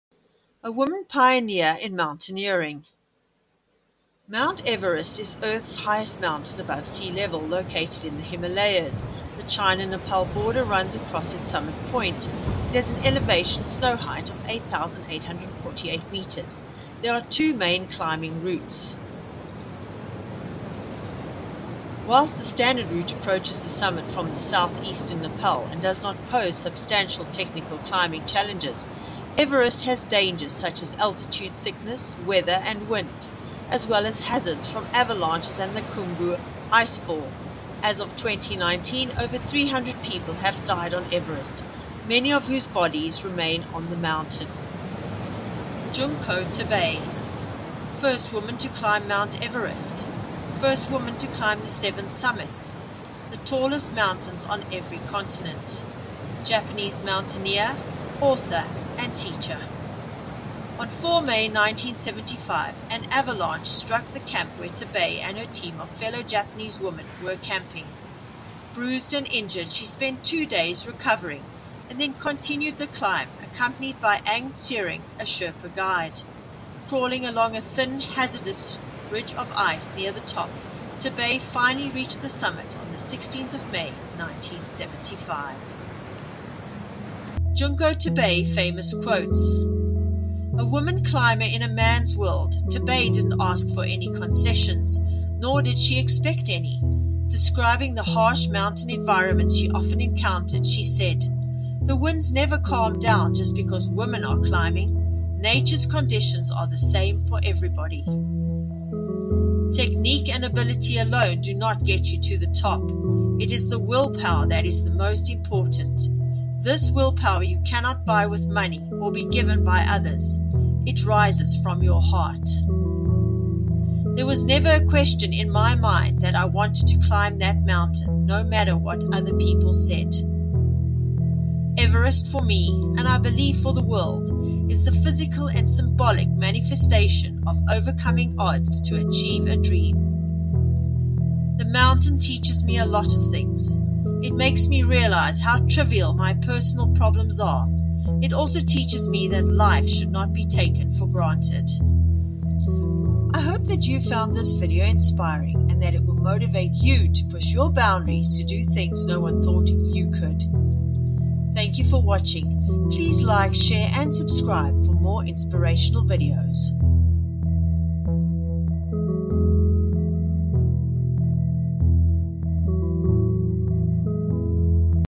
A BRAVE WOMAN PIONEER IN MOUNTAINEERING - JUNKO TABEI
Learn about this remarkable Woman Pioneer's achievements and be inspired by her to achieve your dreams!